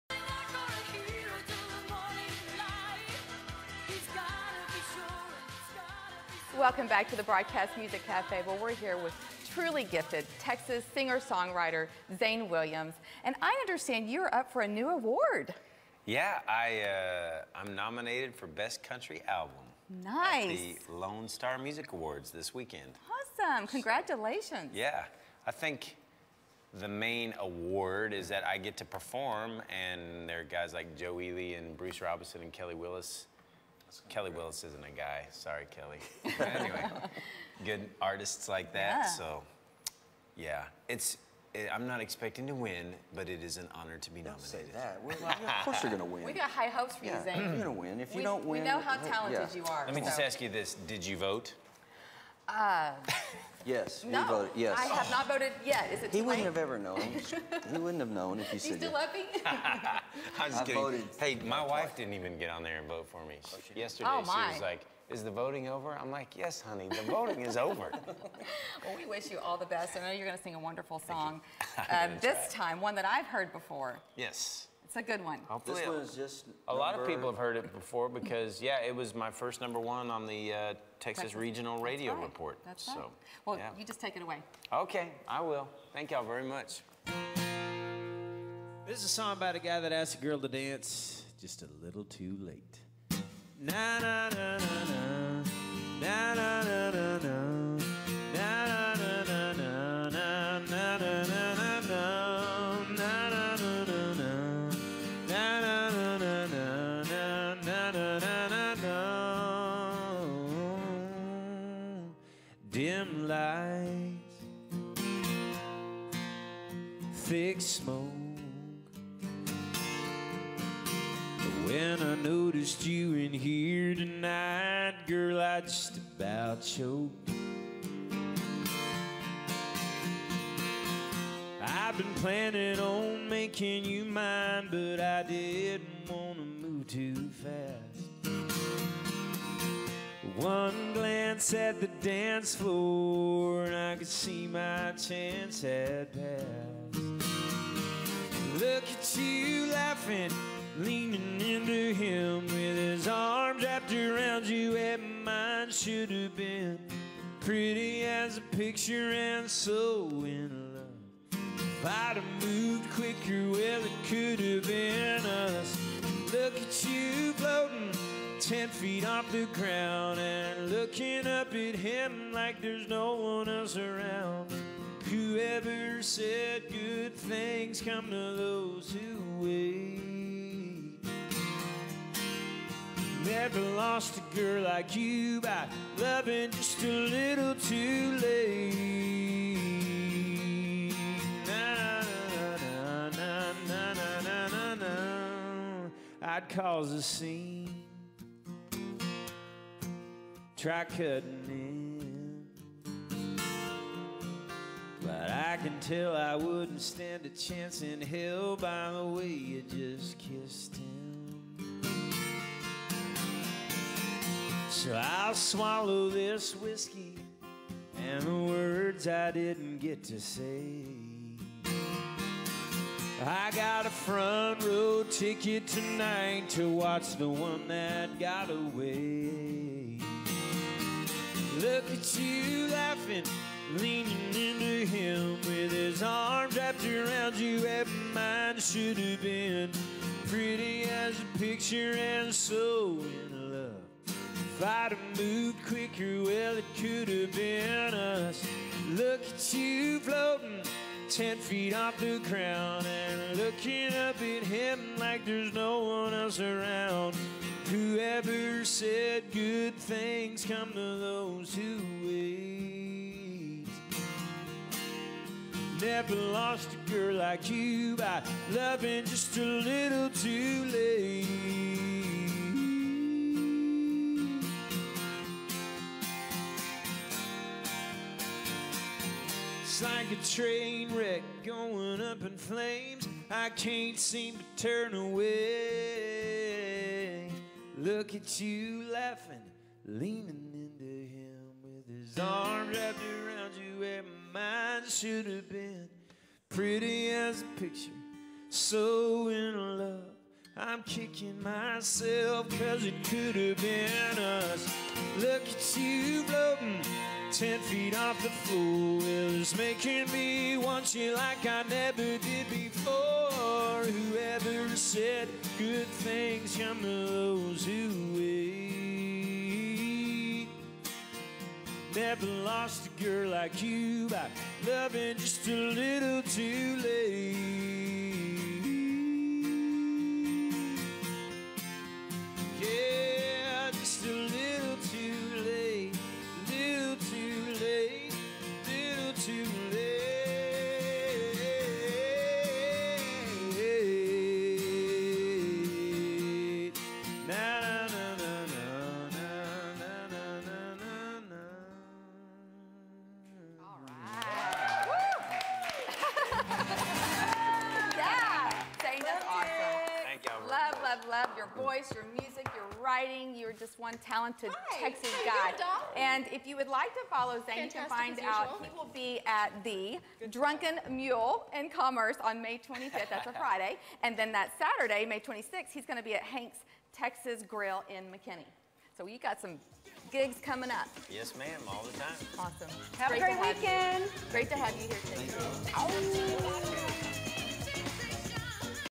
Zane Williams Performs Again
Now we have a couple of the stars of the hit TV series, "Troubadour Texas" here in our studios.